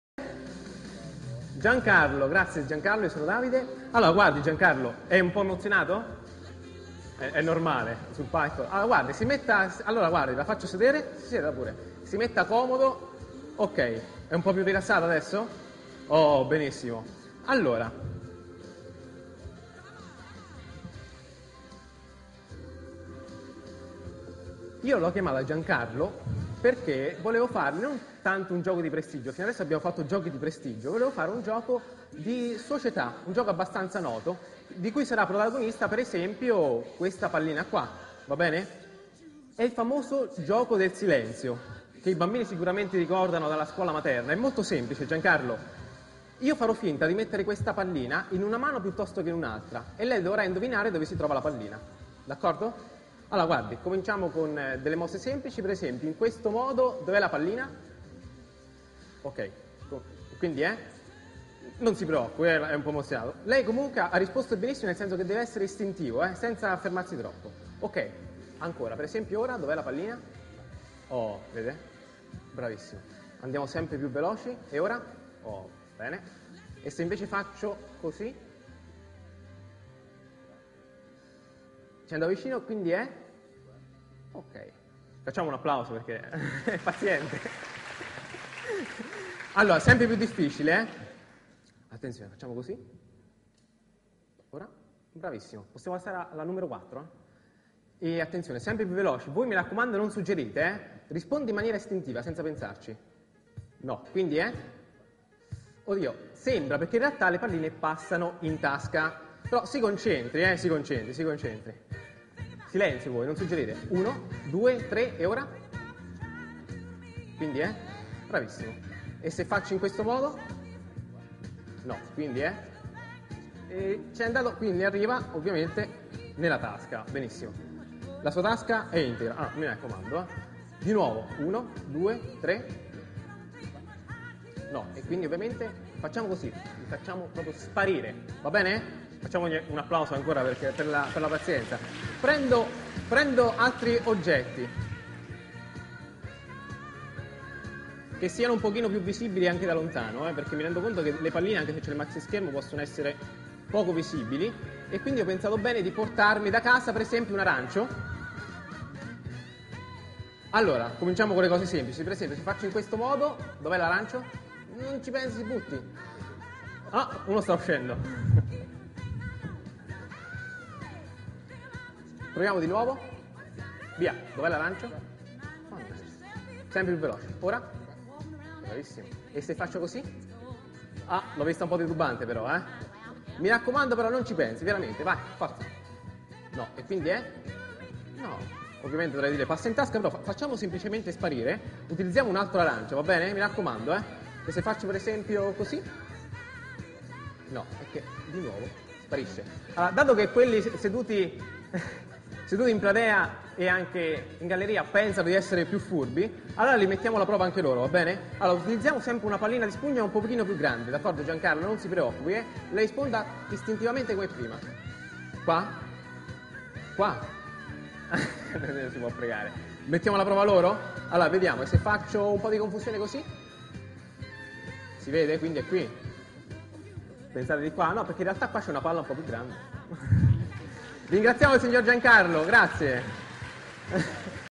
Sponge Balls Gag (2003)
This is a popular trick where only one volunteer, chosen randomly from the audience, doesn't understand how the trick work, while all the others do. Only at the end, the situation reverses: the audience thinks to know the solution but in fact neither the volunteer or the audience know anymore the trick! This performance took place in the theater Verdi of my hometown Terni, Italy, in 2003.